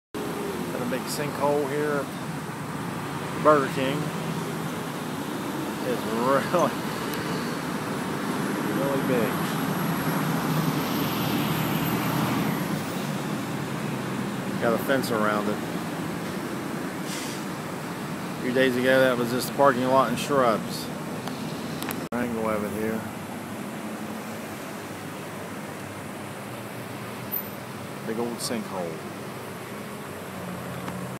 Sinkhole video : at Burger King in Bridgeport West Virginia
Sinkhole video :  at Burger King in Bridgeport West Virginia. It was caused by a collapsed drain pipe